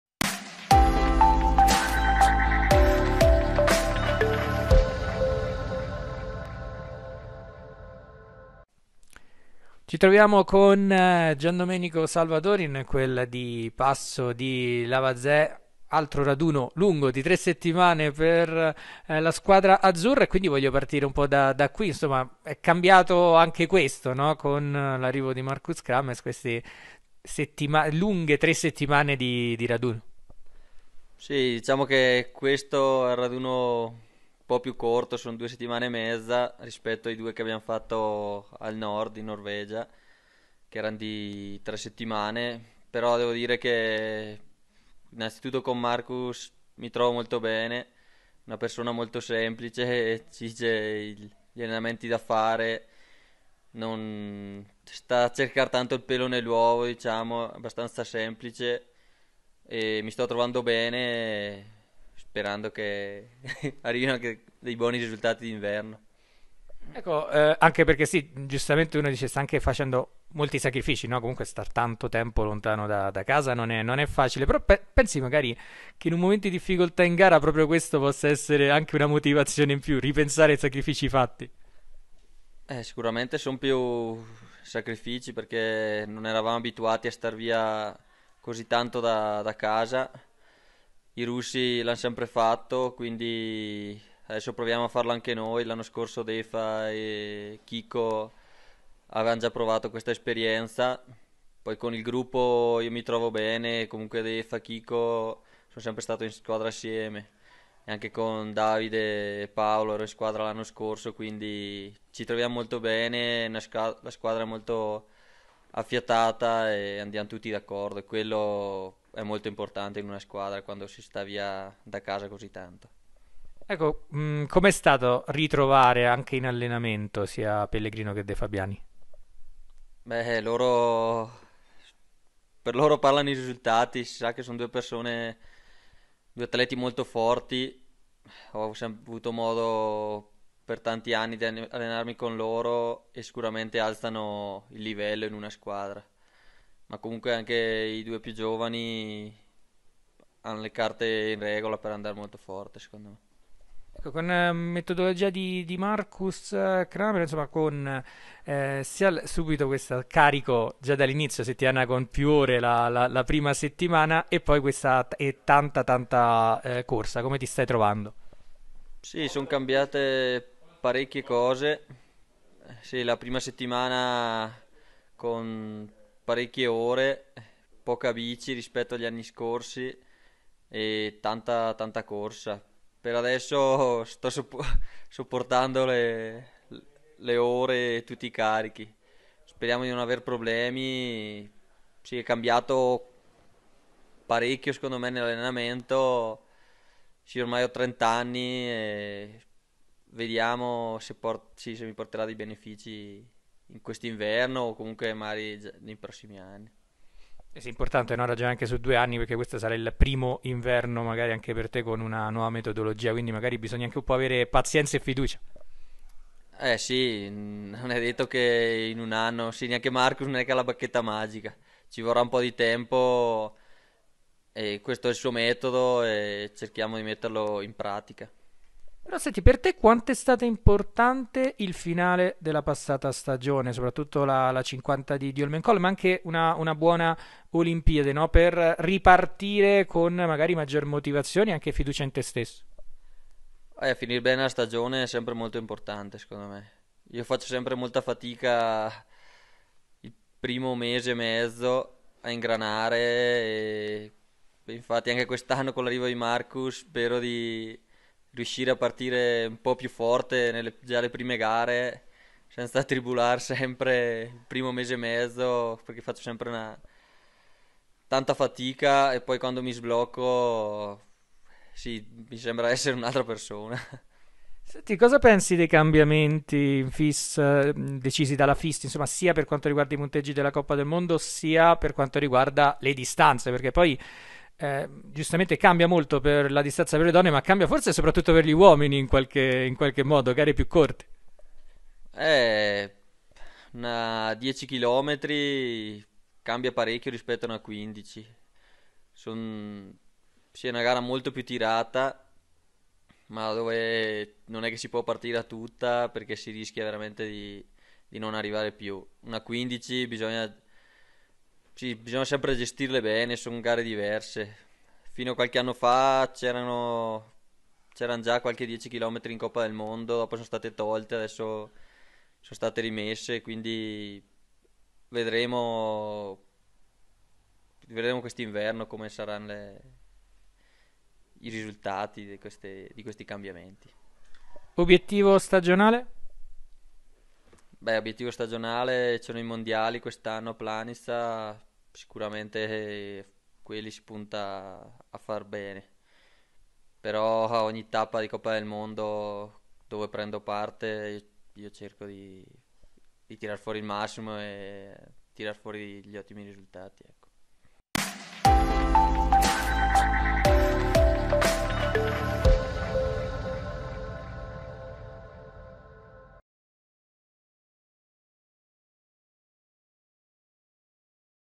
Sci di Fondo - Giandomenico Salvadori: "Con Markus stiamo lavorando tanto"
Fondo Italia ha incontrato Giandomenico Salvadori in occasione del raduno svolto dalla nazionale italiana di sci di fondo a Passo di Lavazé. L'azzurro ha parlato della nuova metodologia di lavoro con Markus Cramer. Intervista di Giorgio Capodaglio.